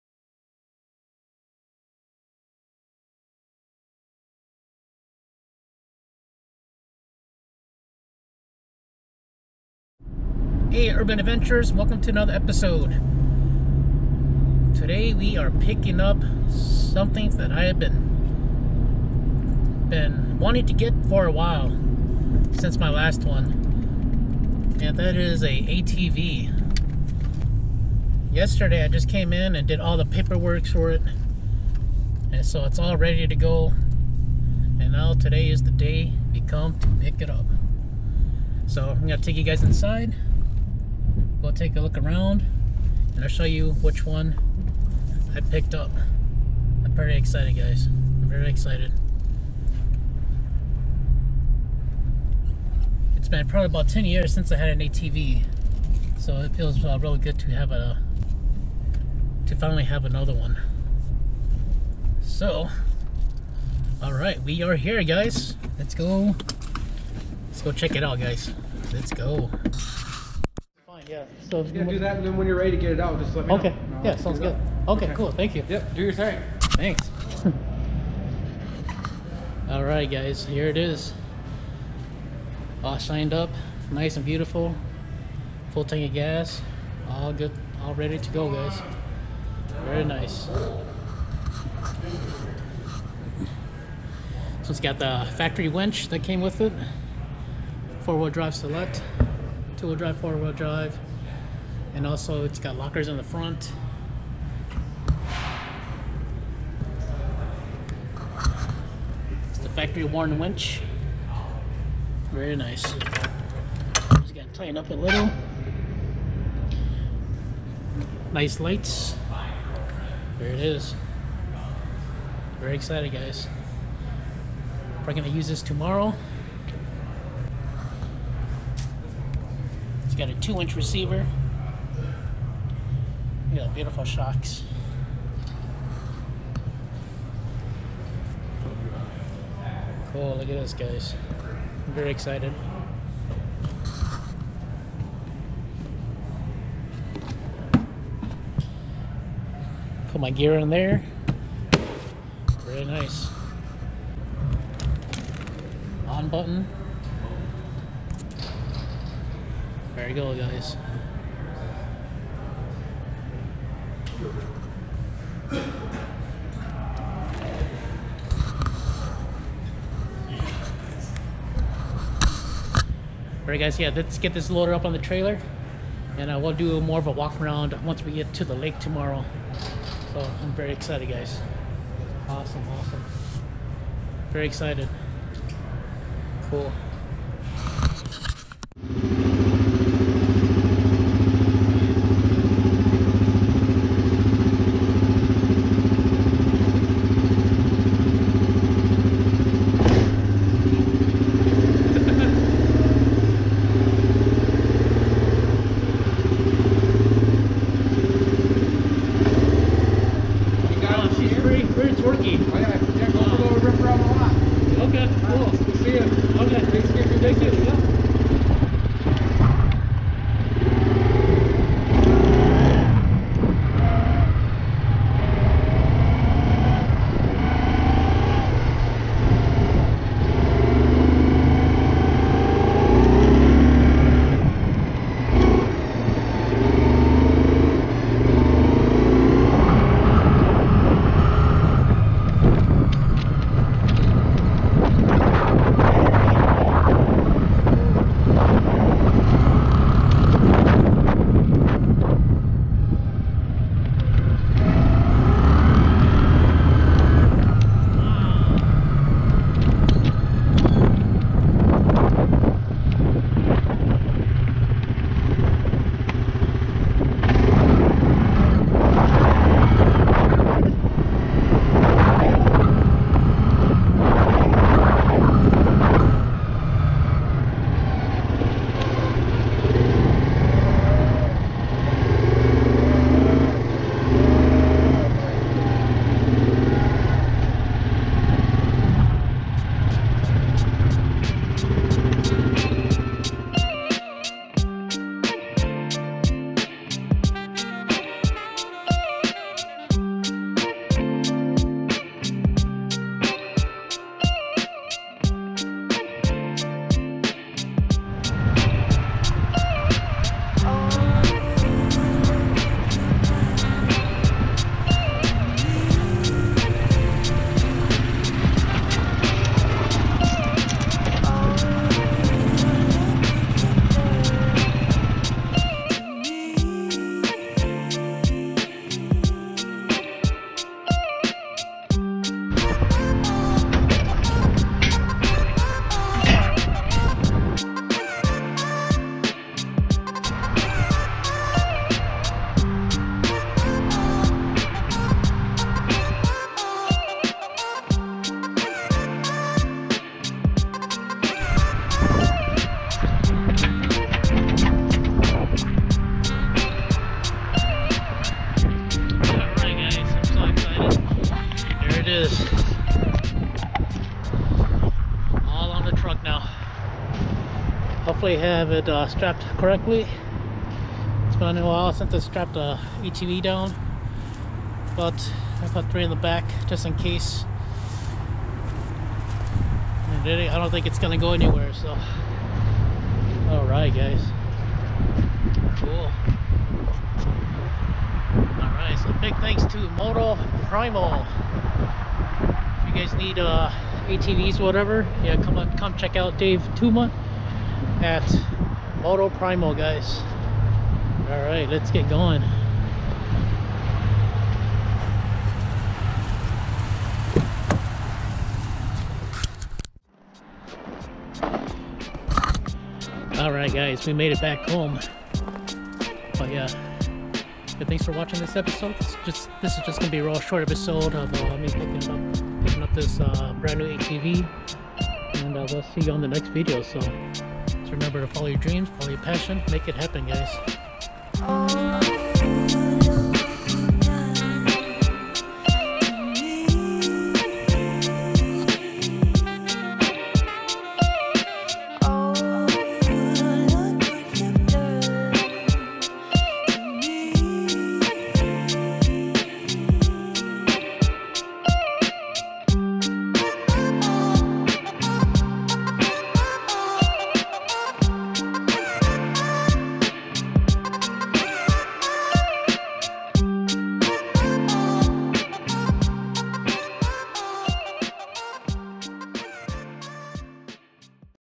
2022 Yamaha Kodiak 700 EPS SE Tactical Black | My New Ride
Picked up my new ATV! Can't believe actually been 18 years since I last owned one! I previous owned a 2004 Honda Rancher 350 ES and had a blast in that ATV! Now on to new adventures with this 2022 Yamaha Kodiak 700 EPS SE! A huge step from the Honda Rancher 350 ES.

Welcome to Wild Urban Adventures: One Adventure at a Time!

All music I use are licensed by Storyblocks.com, Movavi Video Suites and Ceaser Beats.